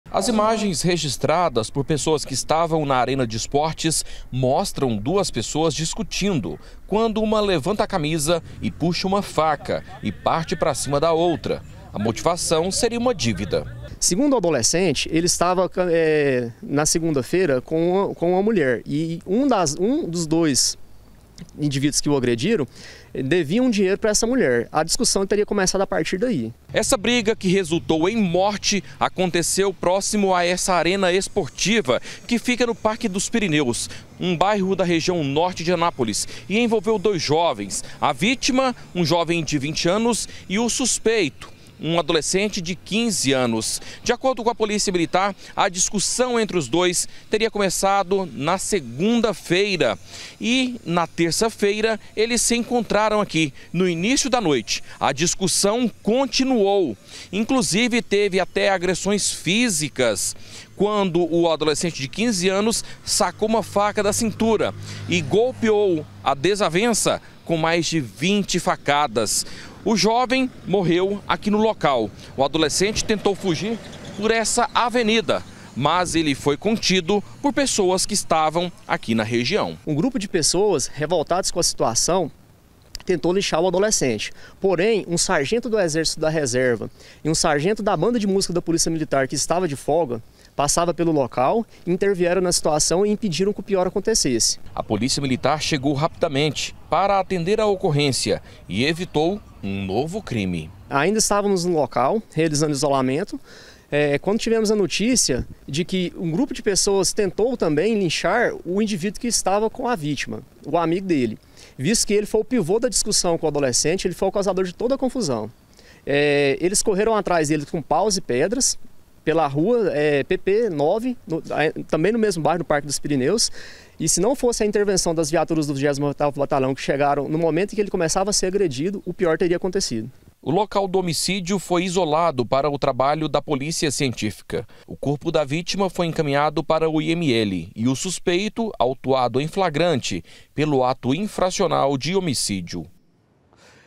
BARBARIDADE: ADOLESCENTE DE 15 ANOS MATA DESAFETO A FACADAS
JOVEM DE 20 ANOS É MORTO COM MAIS DE 20 FACADAS. O SUSPEITO DE TER PRATICADO O CRIME É UM ADOLESCENTE DE 15 ANOS QUE FOI APREENDIDO.